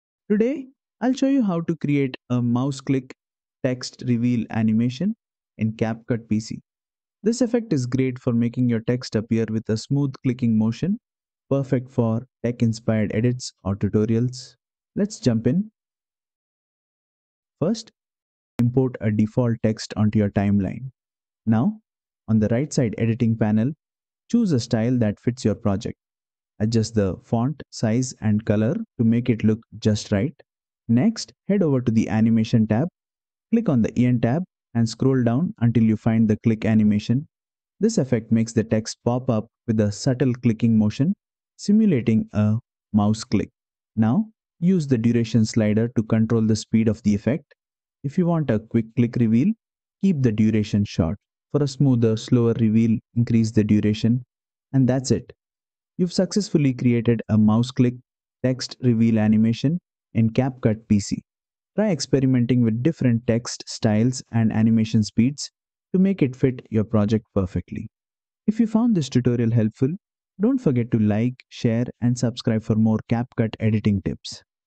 How to Add Mouse Click Text Reveal Animation in CapCut PC Desktop - Lesson 177 CapCut Tutorial
Want to create a cool Mouse Click Text Reveal animation in CapCut PC? In this tutorial, I’ll show you how to use the Click animation effect to make your text appear with a mouse click effect, perfect for tech-style edits and engaging intros!

To do this, go to the Effects tab and import a default text onto the timeline. On the right-side editing panel, choose a style that matches your project. Then, head over to the Animation tab, click on the IN tab, and select the Click animation. Finally, adjust the duration slider to control how long the animation lasts.

And that’s it! Now your text appears with a smooth mouse click effect.